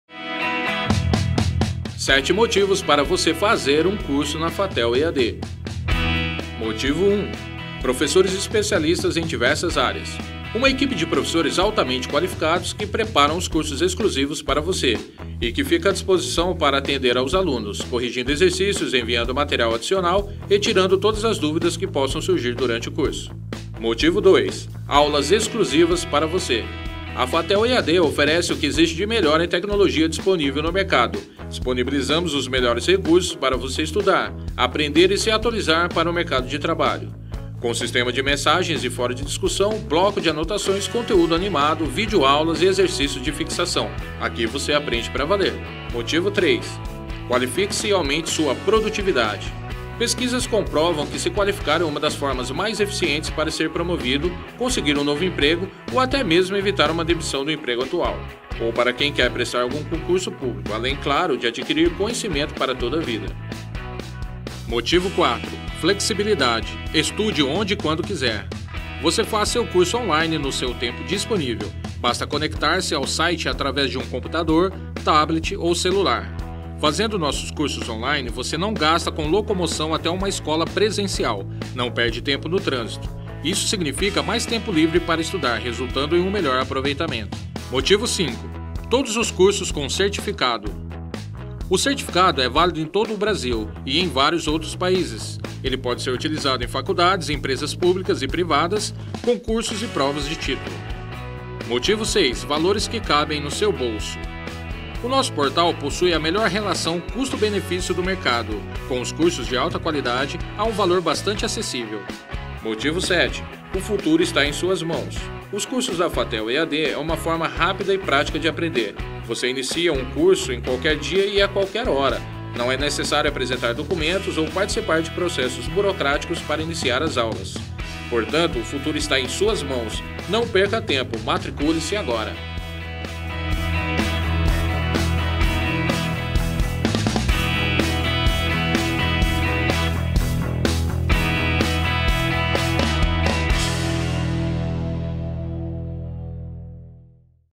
FATHEL EAD - Educação mais perto de você
O Portal EAD da FATHEL visa levar conhecimento em diversas áreas, por meio de cursos de curta duração, valor acessível e formação permanente e personalizada. A plataforma de aprendizado oferece videoaulas, apoio com materiais digitais, questionários e certificação ao término do curso. Com esta ferramenta virtual, o aluno passa a ser sujeito ativo na construção de sua aprendizagem, possibilitando autonomia e flexibilidade de horários no qual se pode estudar com acesso 24 horas por dia.